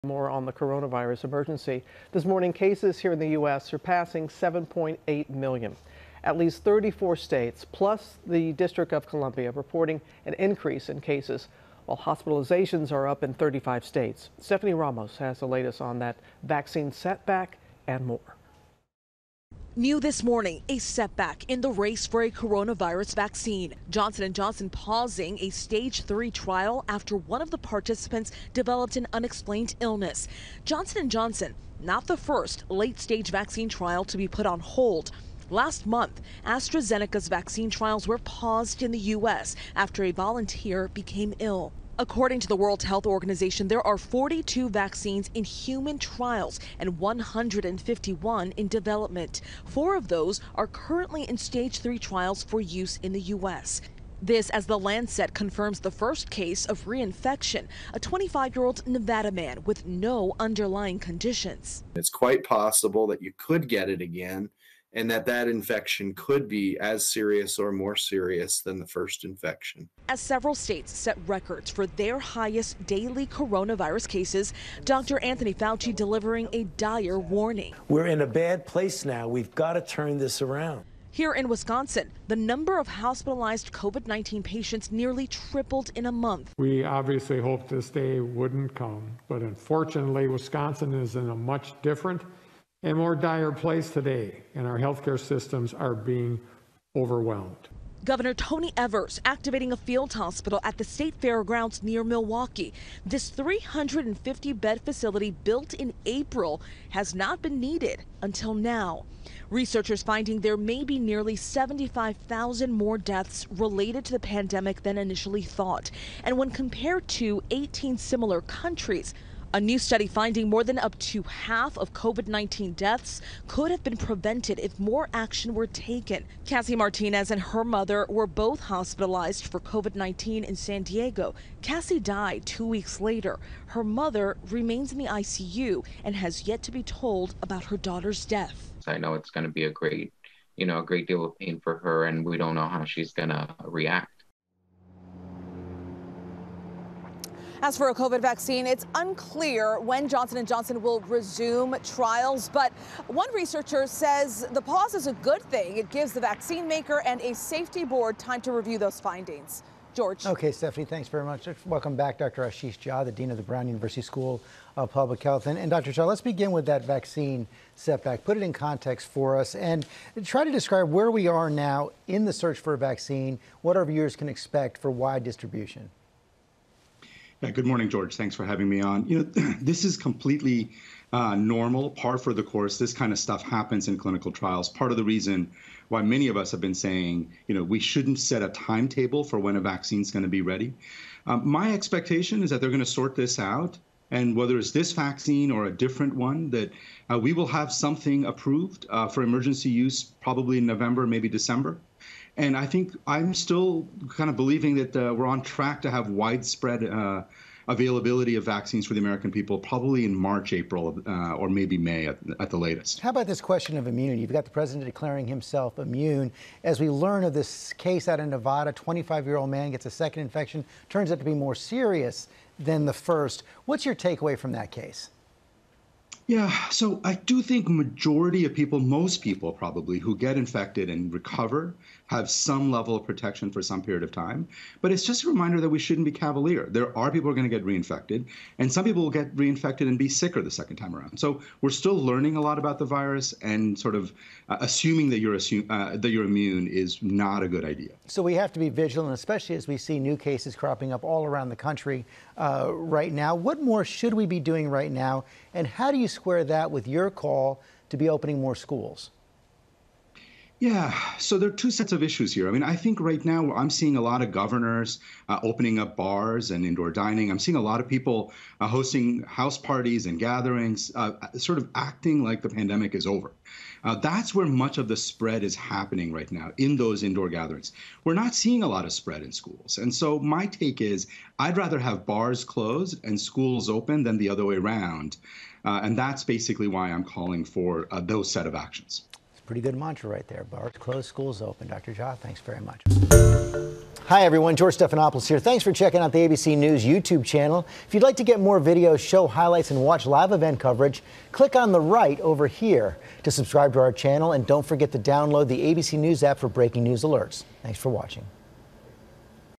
Coronavirus vaccine setback as researches confirm case with repeat infection
Johnson and Johnson paused the stage 3 trial after a participant developed an unexplained illness as the Lancet confirmed reinfection in a 25-year-old Nevada man without underlying conditions

Judge strikes down attempt to stop mask mandate